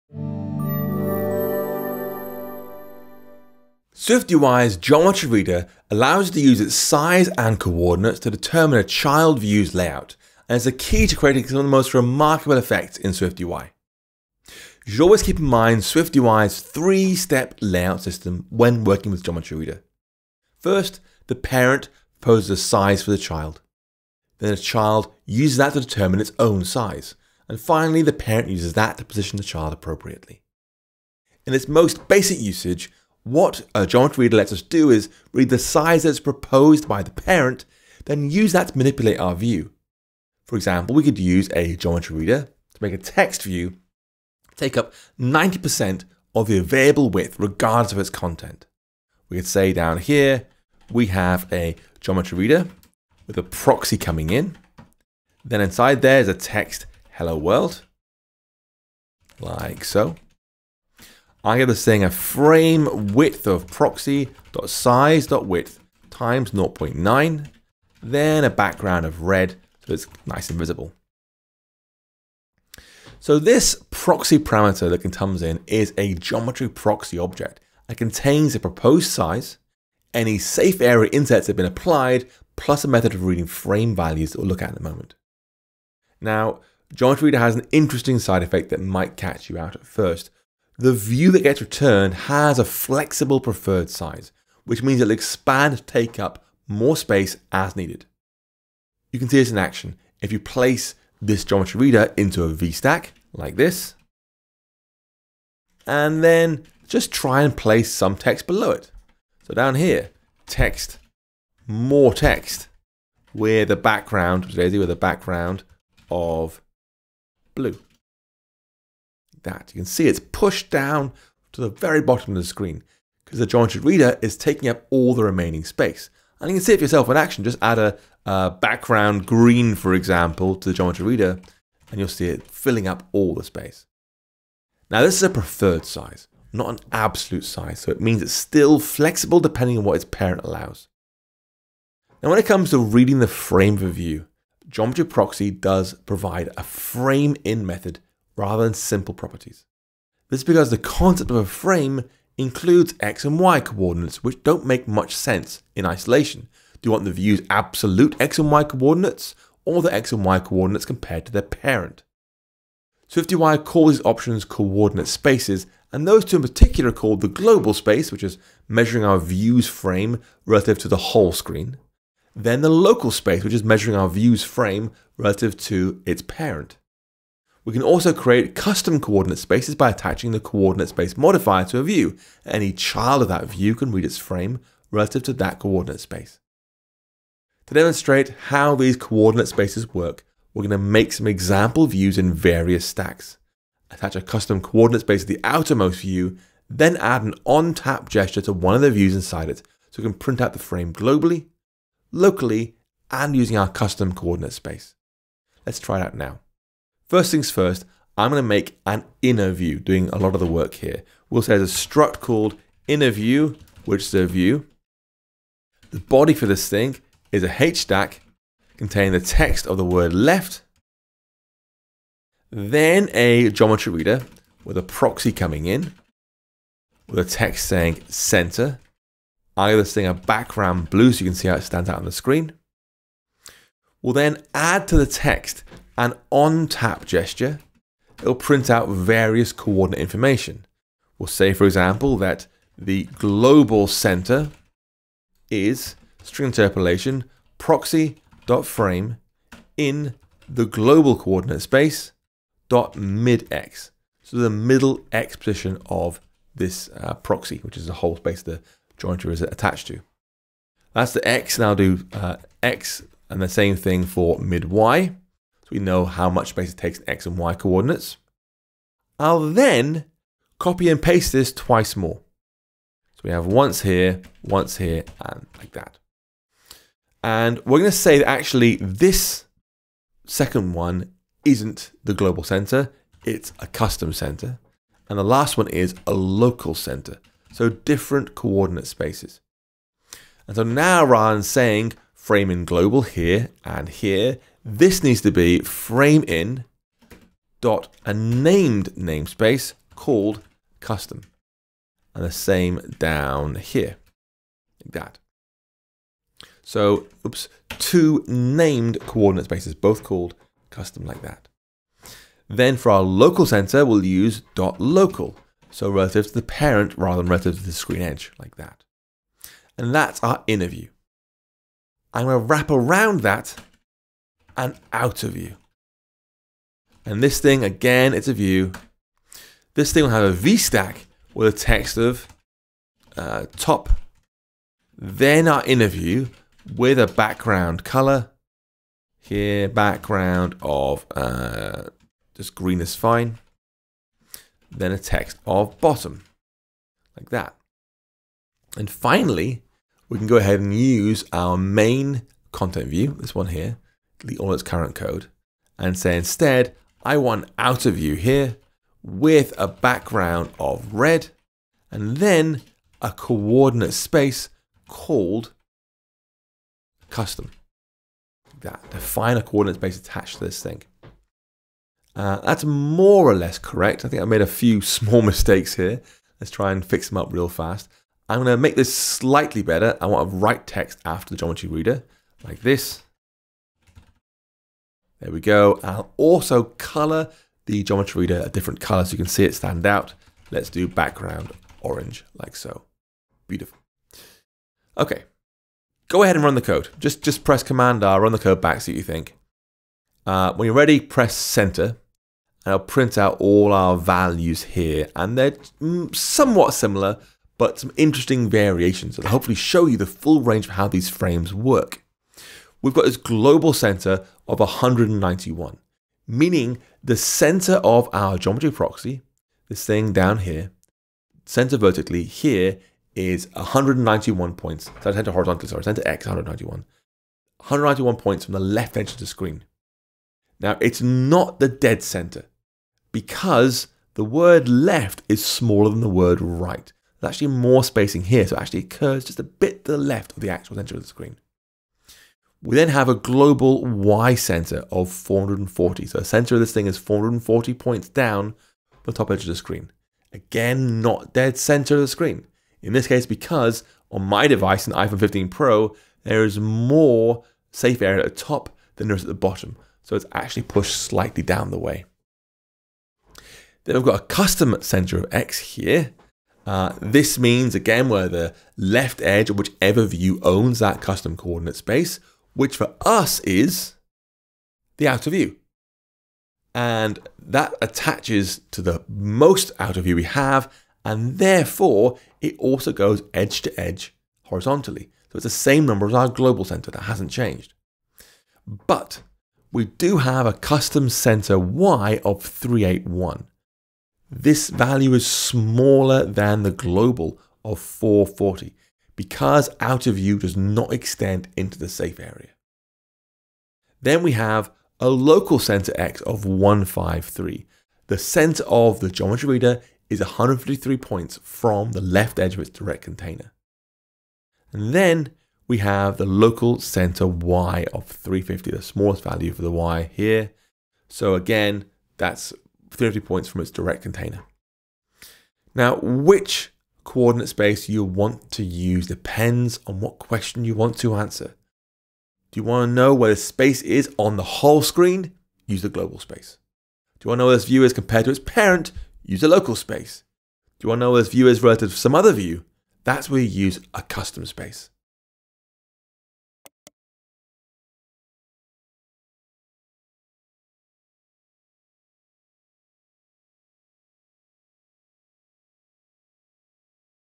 Understanding frames and coordinates in GeometryReader – Layout and Geometry SwiftUI Tutorial 6/8
6. Understanding frames and coordinates inside GeometryReader: This video